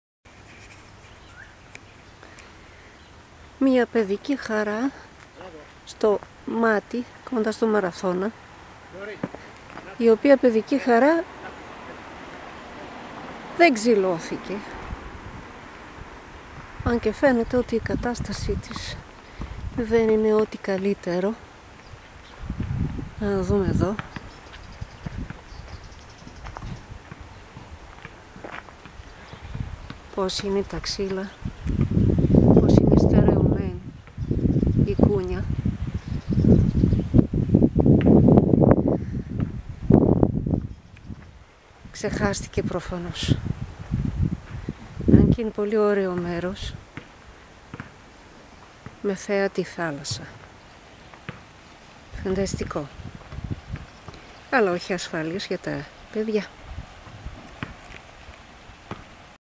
Marathon Greece. 2018 A Dangerous Playground
Στο Μάτι κοντά στο Μαραθώνα ξεχάστηκε μια παιδική χαρά ... Φανταστική τοποθεσία αλλά επικίνδυνη παιδική χαρά... Προφανώς όλοι οι Δήμοι δεν είχαν τον ίδιο ζήλο στο να εφαρμόσουν το νόμο..